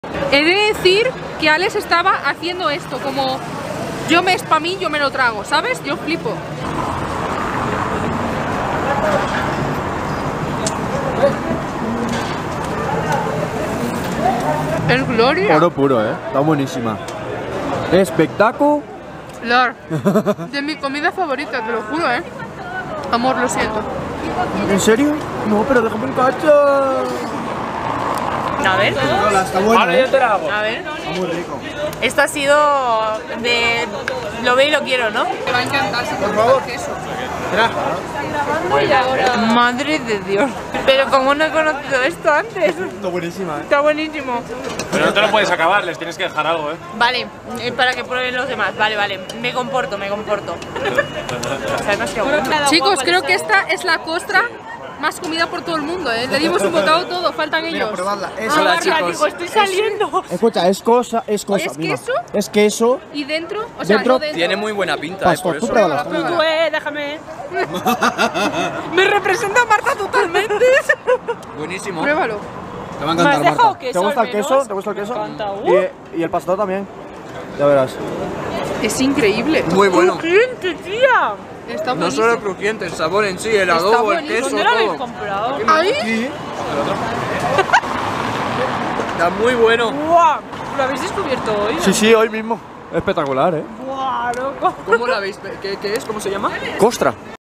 Españoles prueban costra de queso
Españoles enloquecen al probar costra de queso en México Comeremos tacos mexicanos olé